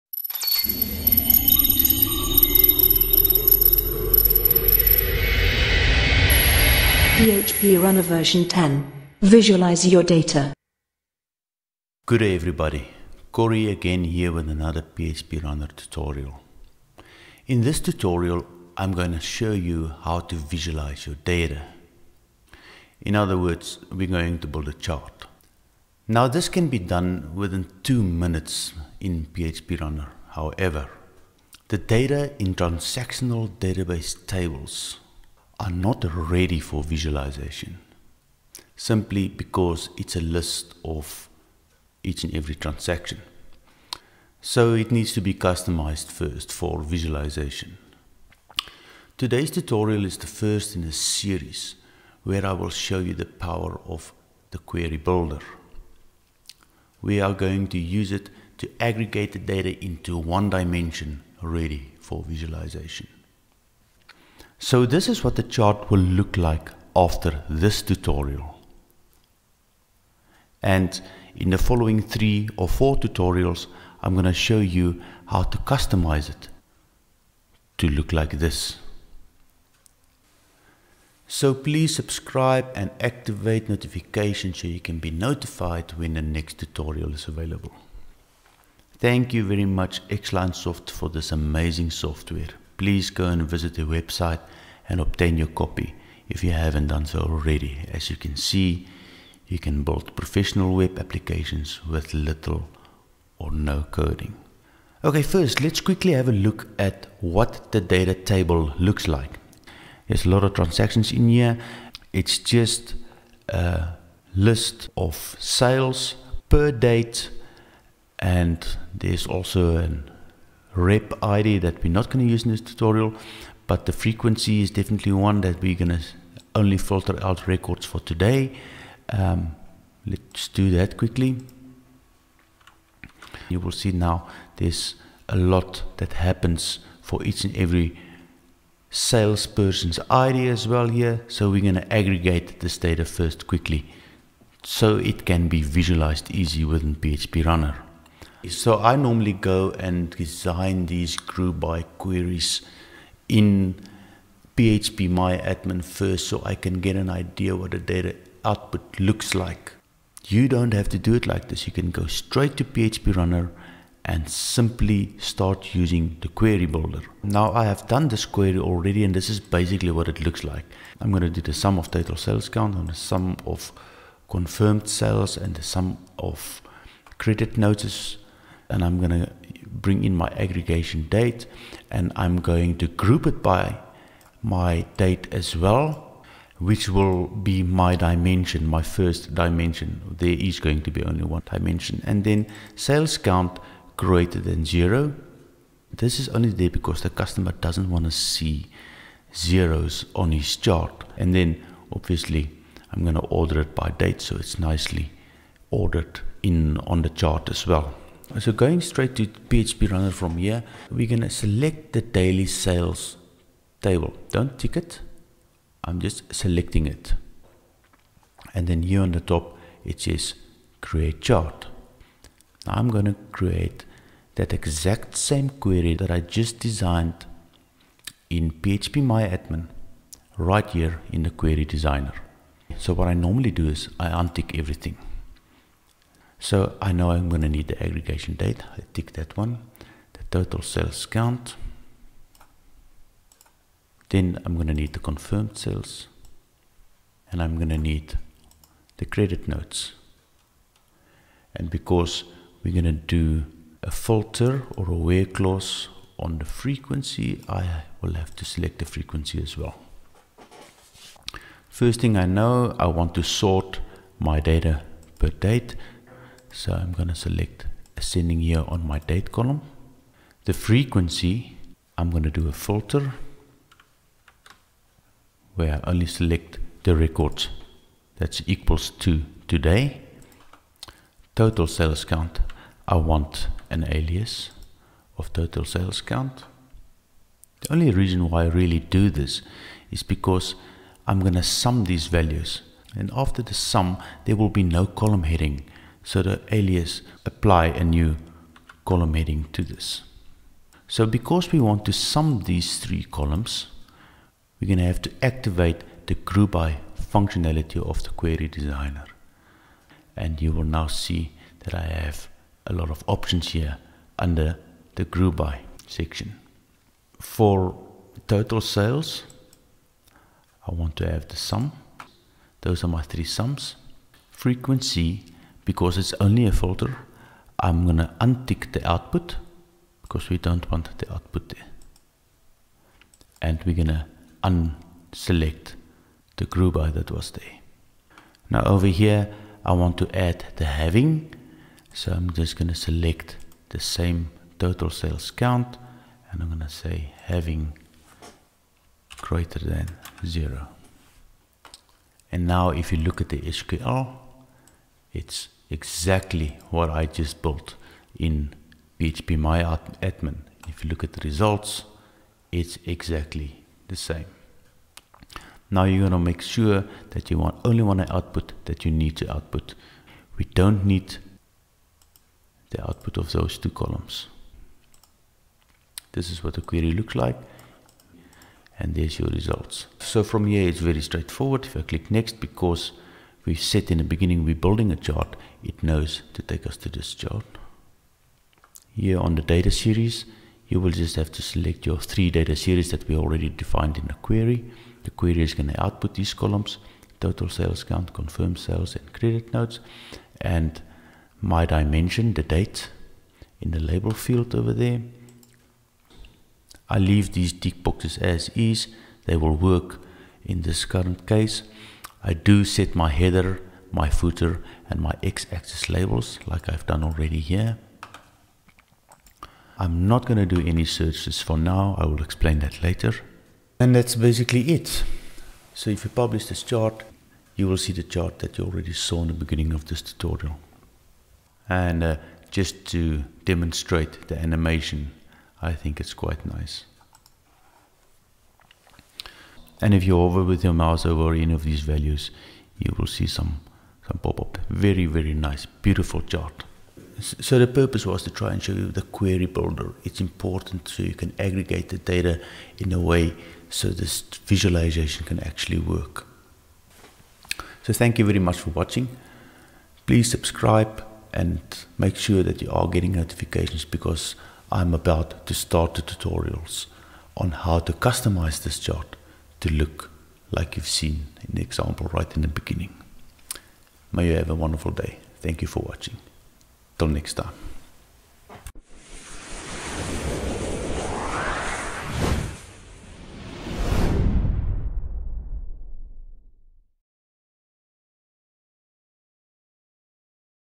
PHP Runner V10.1 - Prepare the data for visualization
In this tutorial, I am going to show you how to visualize your data.
The data in transactional database tables are not ready to be used for visualization. Here I show you how to aggregate the data within the query designer of PHP Runner version 10.1